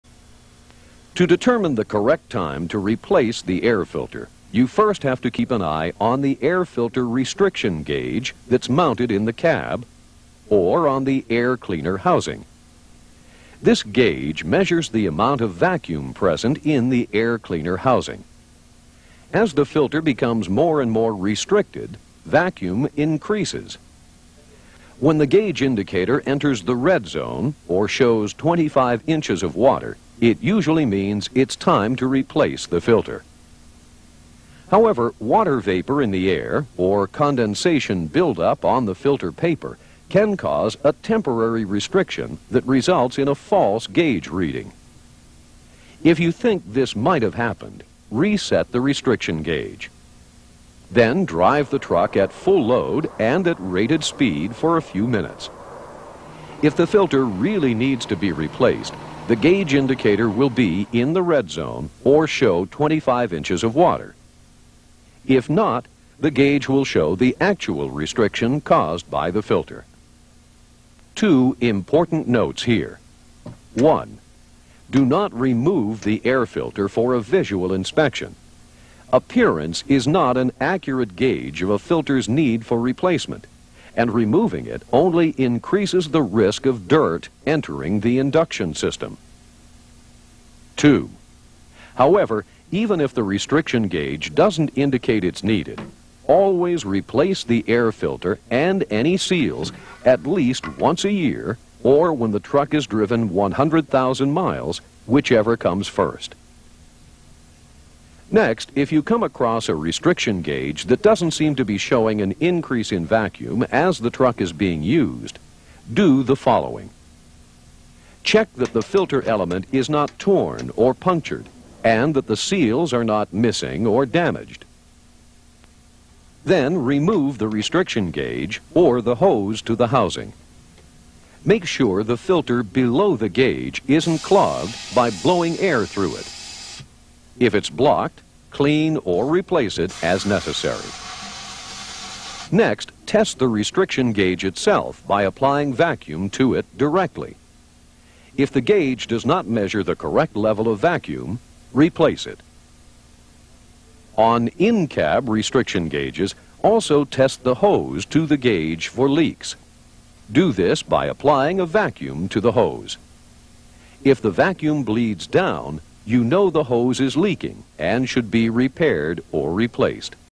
Air Filter Restriction Gauge Operation & Testing
Excerpt from Mack Truck video for air filter restriction indicator inspection and testing on trucks and diesel engines.